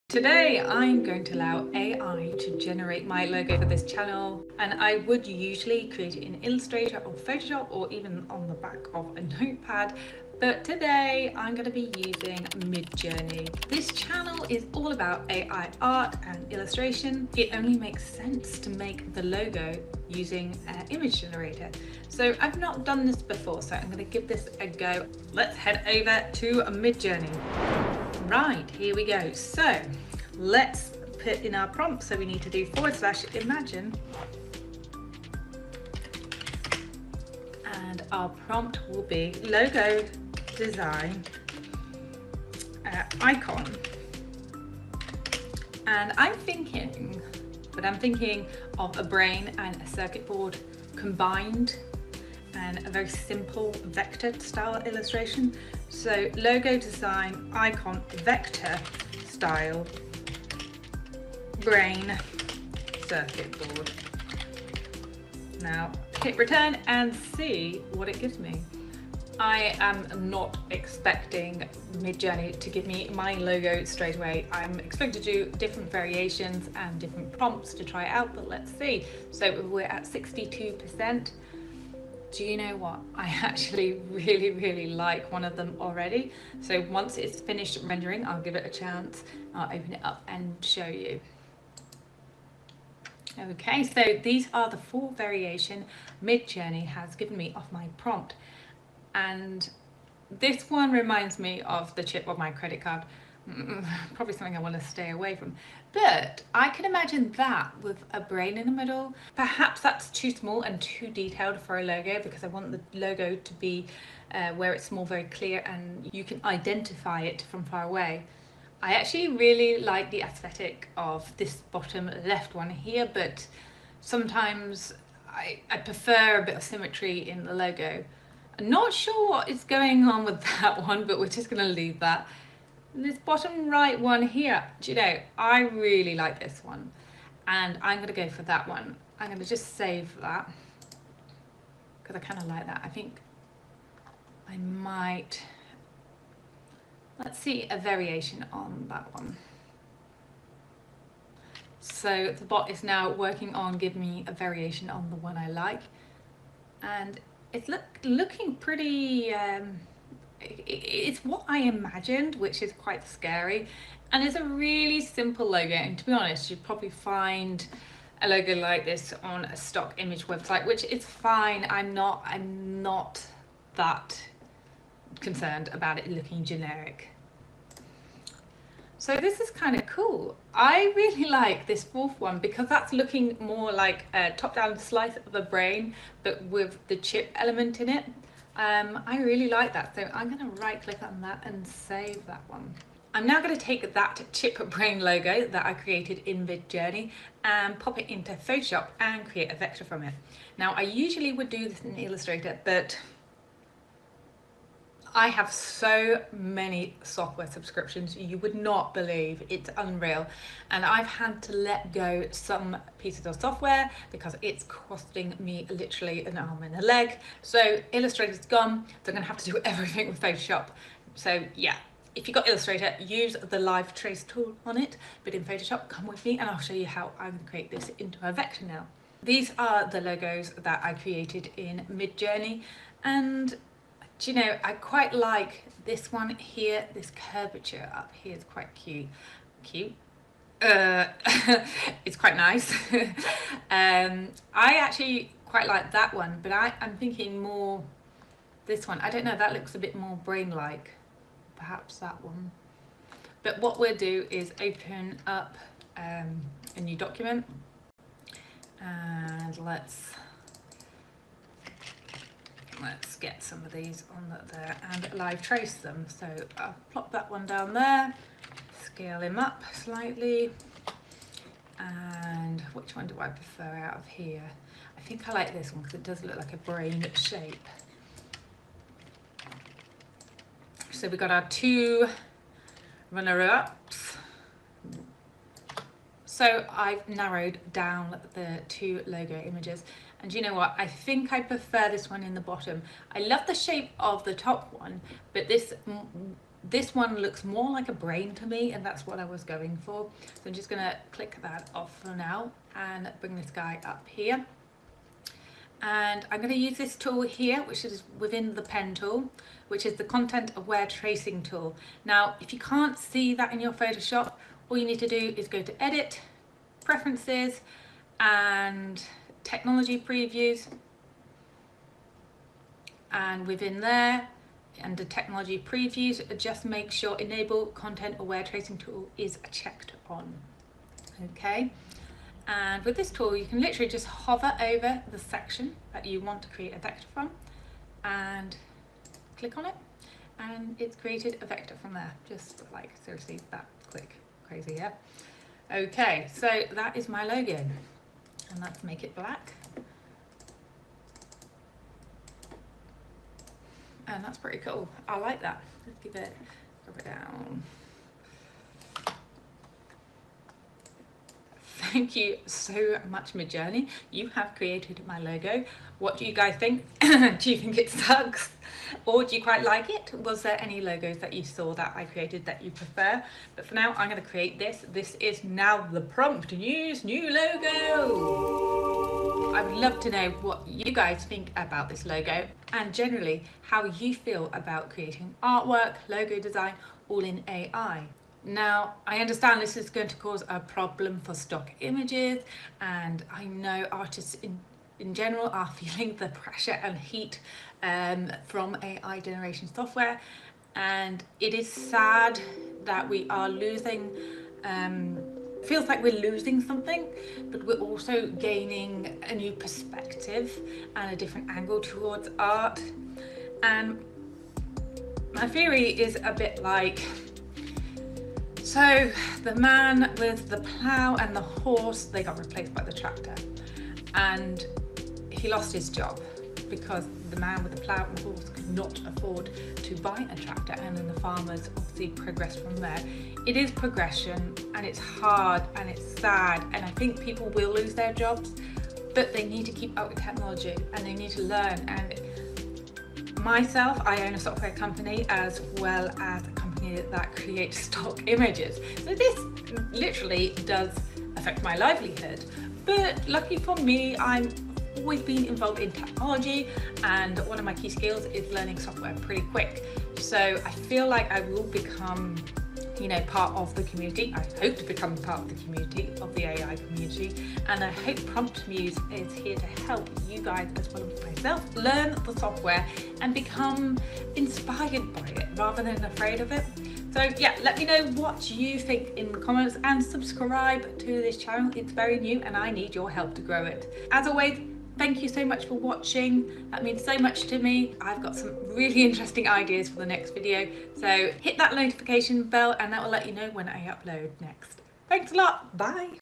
How A.I. Designed my logo for Free
I wasn't sure what to expect from an A.I.generated logo and wanted to explore what it could do with one simple Prompt.

 I'm really happy with the result, I wanted a super simple logo and Midjourney really impressed me. (Especially being free).

I also show how to then take your logo and turn it into a vector in Photoshop (Super easy!)

Did you like the logo Midjourney created for me?

If you want to get started with Midjounrey I've created this super easy to follow video for you: